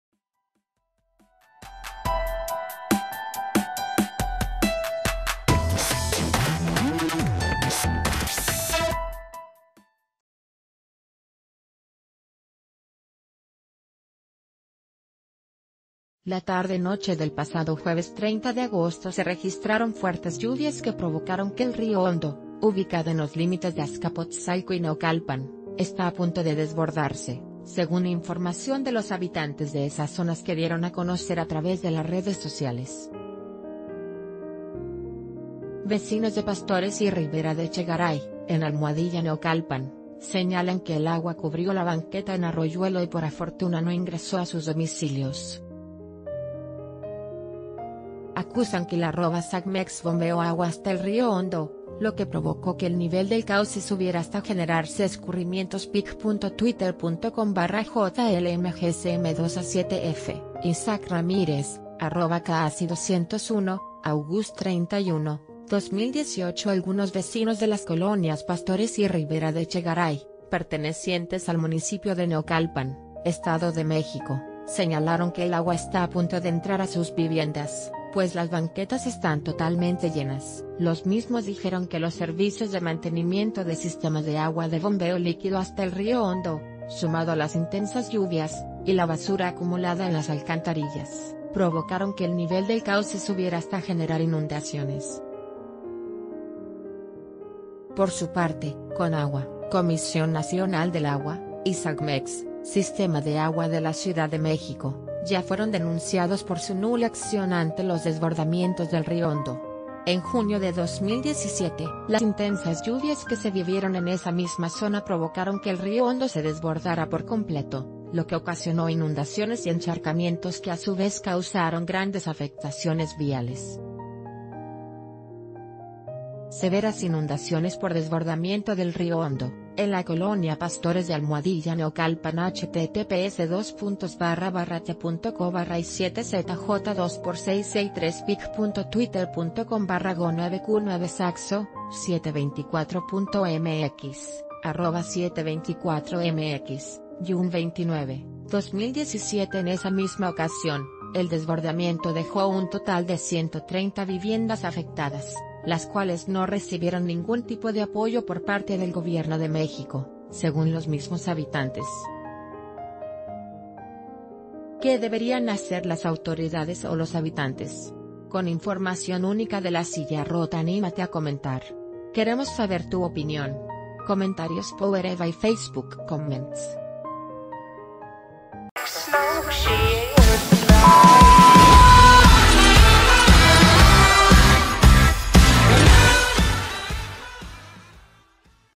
Prevén posible desbordamiento del Río Hondo en Edomex tras fuertes lluvias
La tarde noche del pasado jueves 30 de agosto se registraron fuertes lluvias que provocaron que el Río Hondo, ubicado en los límites de Azcapotzalco y Naucalpan, está a punto de desbordarse; según información de los habitantes de esas zonas que dieron a conocer a través de las redes sociales. Vecino...